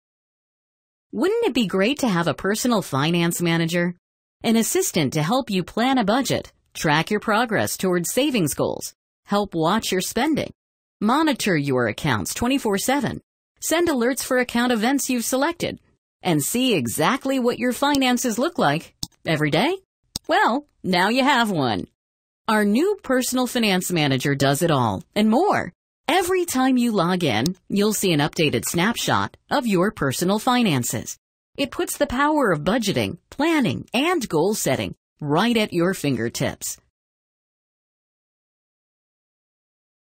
Trends - PFM
Now you can get a complete picture of your finances by bringing the information from all your accounts into our easy-to-use Trends-Personal Finance Manager. Our powerful software allows you to collect your account data from other financial institutions, credit cards, investment accounts, mortgage and insurance accounts into one place. Then, with all your information at your fingertips, our tools help you analyze your spending, track your investment portfolios, reach savings goals and take control of your finances.